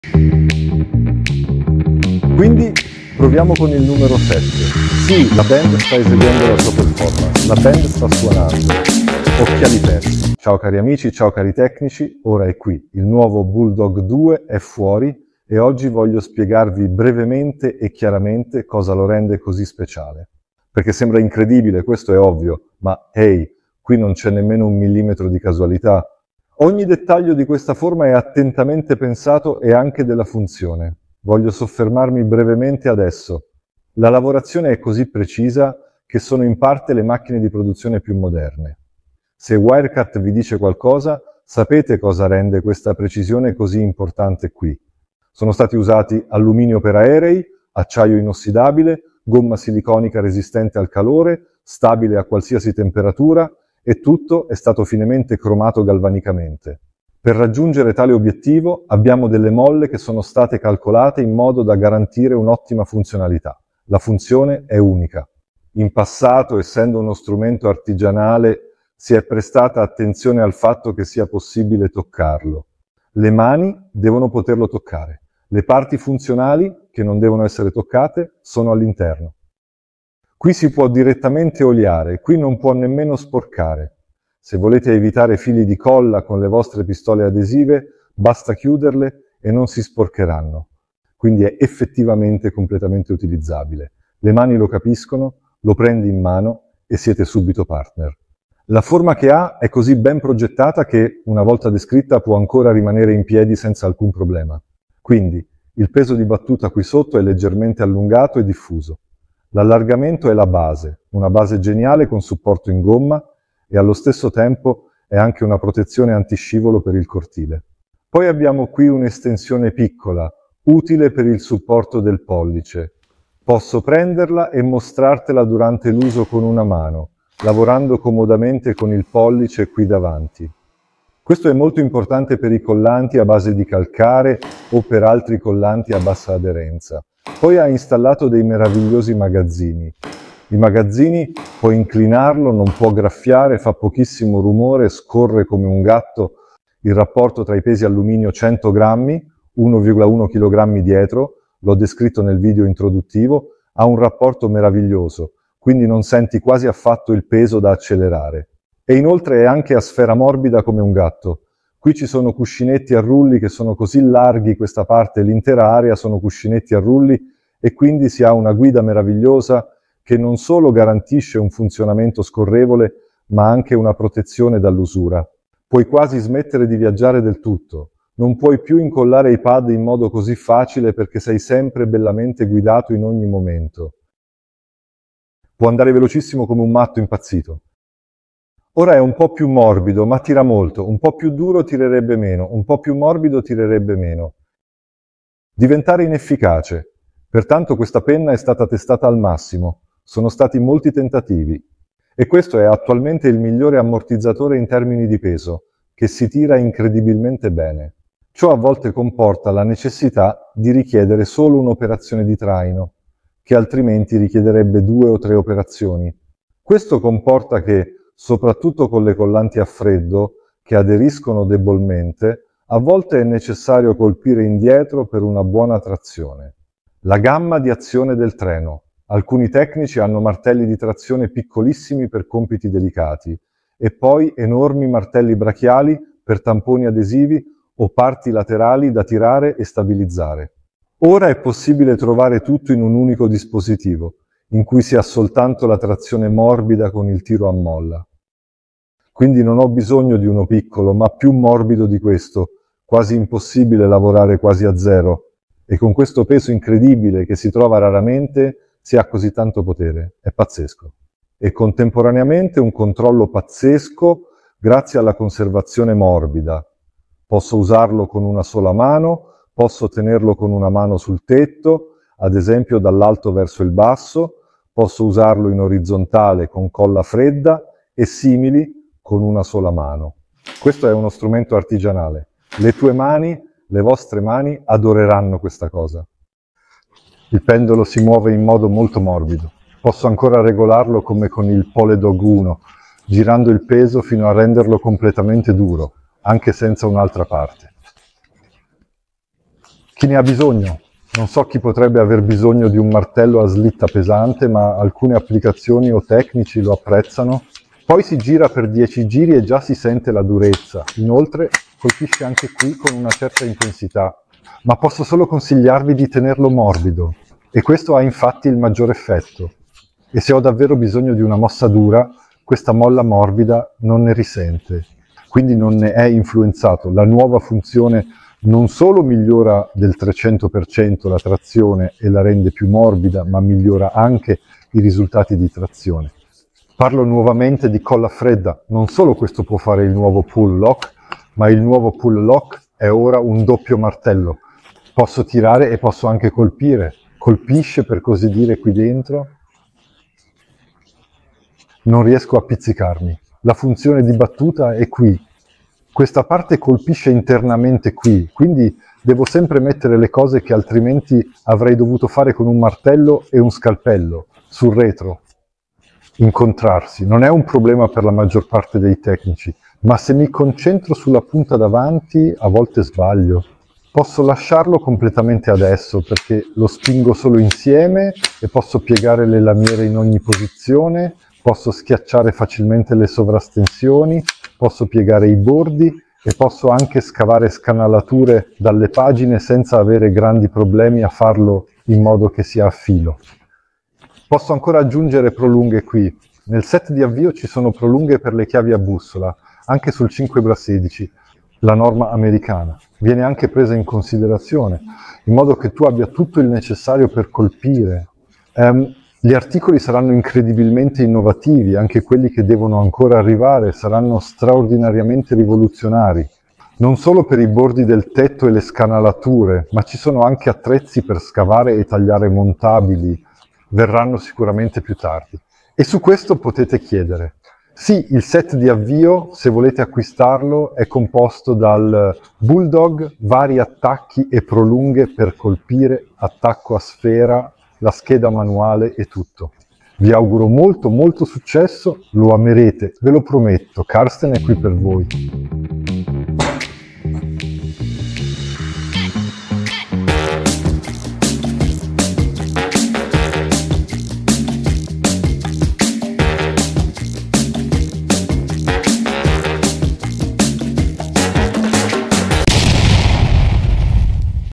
💪🚗🚀 Presentazione del nuovo Martello Scorrevole PullDog II, PDR
Scopri il nuovo Martello Scorrevole PullDog II. Maggiori informazioni su PullDog II 👉👉👉 qui

Questo video illustra la struttura e le possibilità di utilizzo del Martello Scorrevole PullDog II.

Il nuovo PullDog II è il successore avanzato del martello scorrevole PullDog, progettato specificamente per i professionisti del ripristino di ammaccature, in particolare per la riparazione dei danni da grandine.

Grazie alla sua maggiore forza d'impatto, il nuovo PullDog II è efficace anche per i danni più estesi causati dalla grandine, le ammaccature nei parcheggi e i piccoli urti. Il PullDog II può essere utilizzato sia con colla a caldo che a freddo.

Inoltre, il PullDog II può essere utilizzato sia per tirare che per colpire, combinando così due strumenti in uno e stabilendo nuovi standard nel settore.